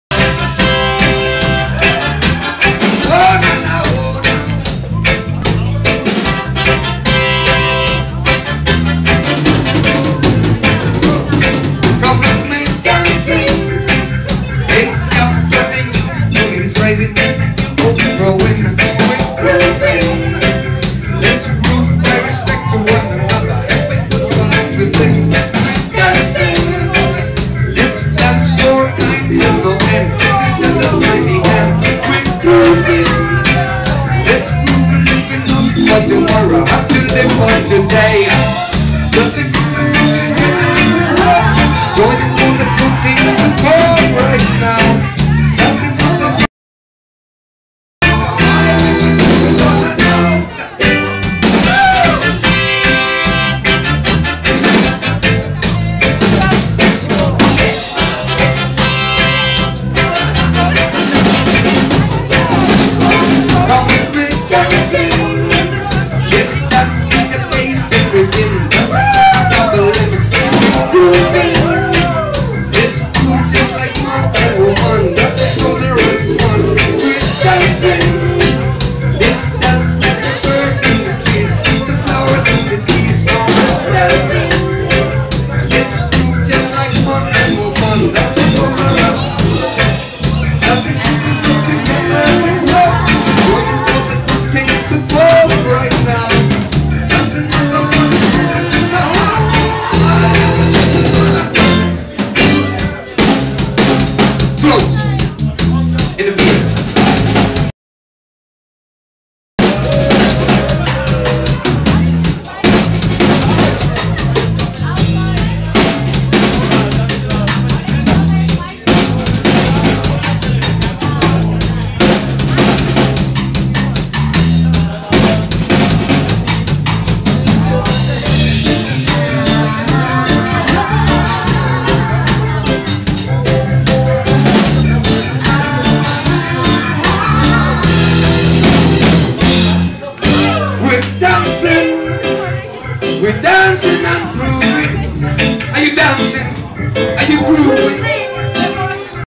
SOUL FIRE - DANCING & GROOVING LIVE AT CLUB LEBOWSKI BULGARIA PLOVDIV 2010
SOUL FIRE DANCING & GROOVING LIVE AT CLUB LEBOWSKI PLOVDIV NEXT DAY AFTER BOB MARLEY'S EARTHDAY CELEBRATION 2010 IN SOFIA BULGARIA